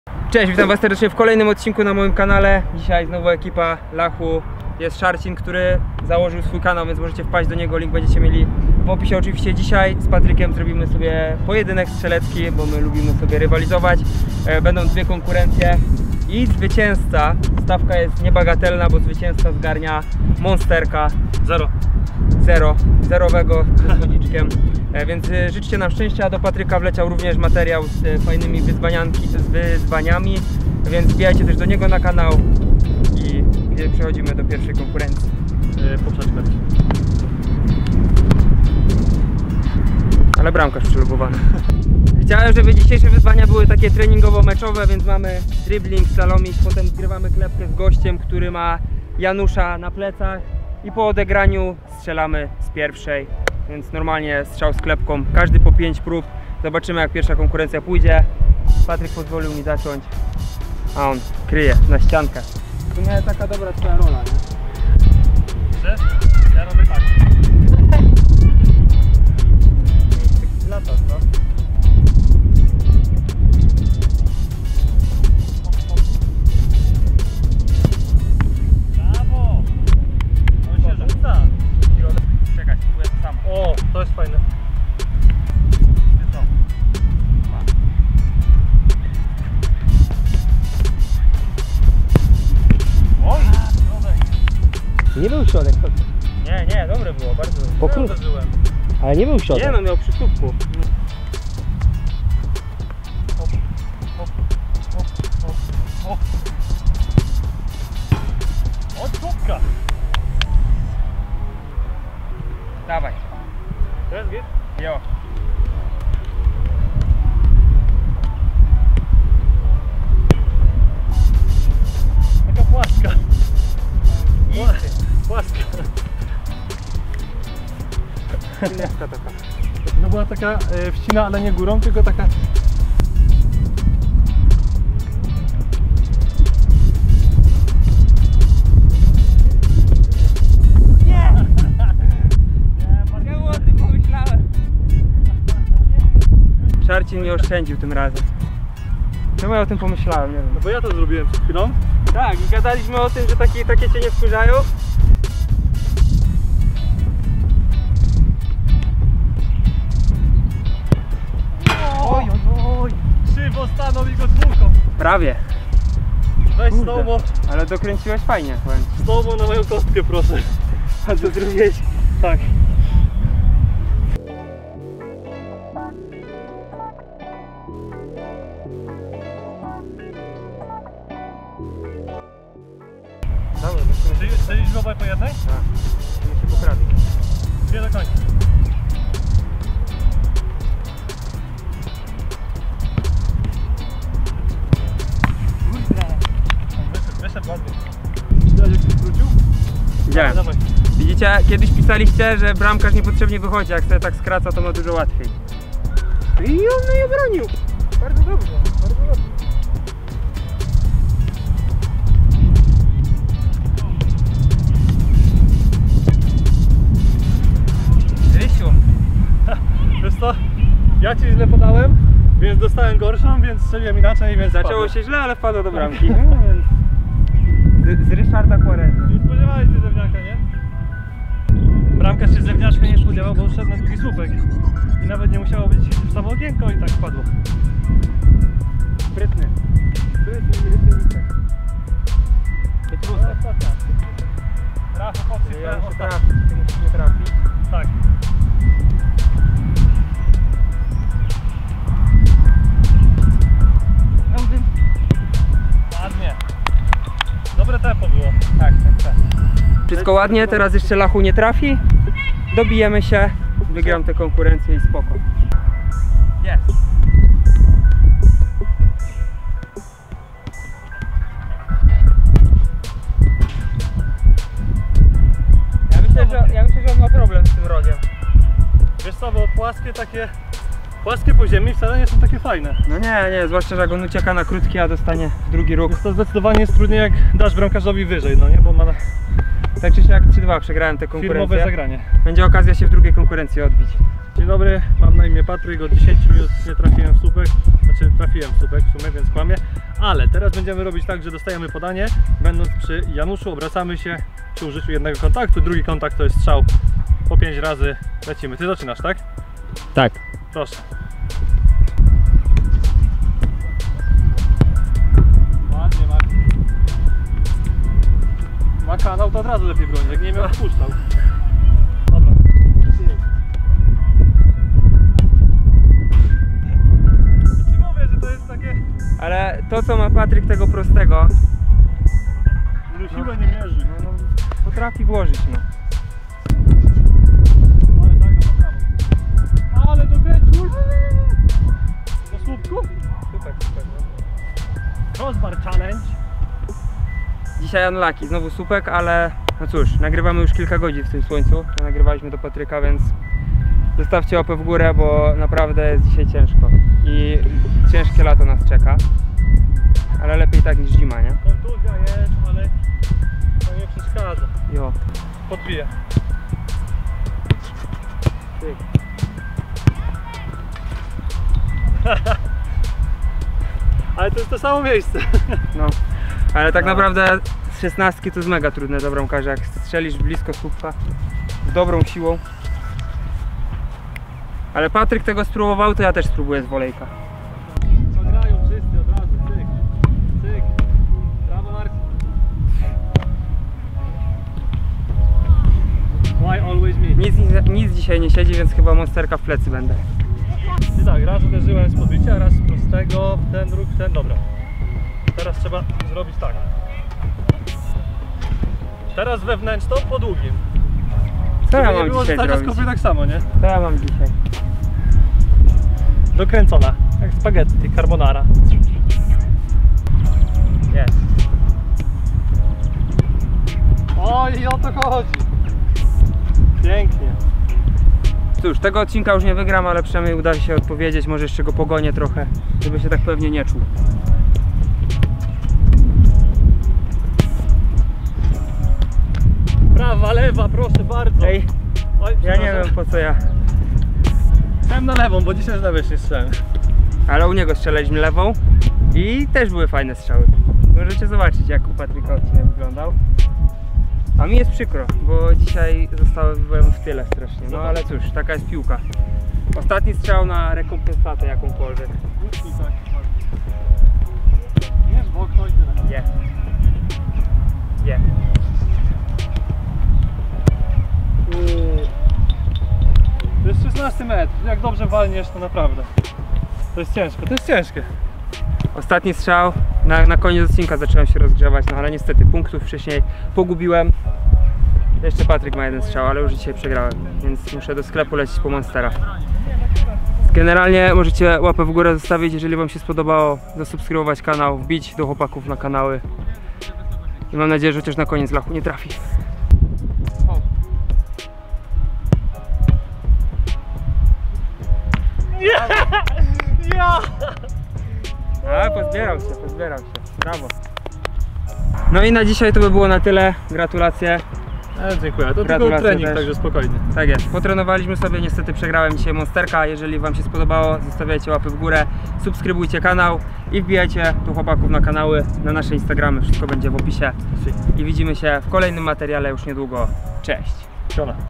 Piłkarski trening strzelecki VS Lachu
►10% Rabat w sklepie R-GOL.com z kodem: Lisu10

Dzisiaj wraz z Lachem w ramach pojedynku zrobiliśmy trening strzelecki o monsterka, a między słupkami powstrzymywał nas Szarcin ;)